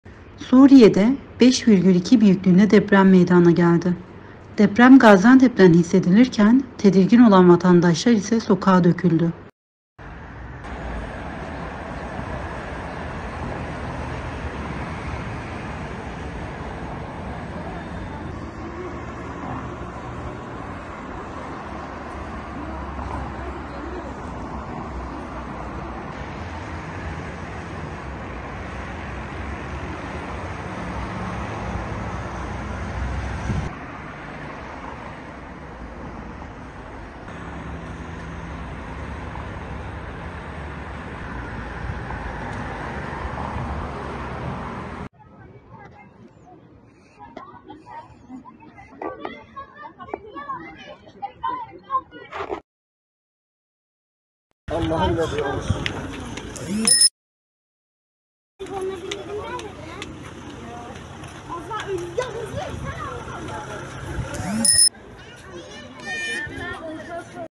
Suriye’deki 5,2’lik Deprem Gaziantep’te Hissedildi
Suriye’de 5,2 büyüklüğünde deprem meydana geldi. Deprem Gaziantep’ten hissedilirken tedirgin olan vatandaşlar ise sokağa döküldü.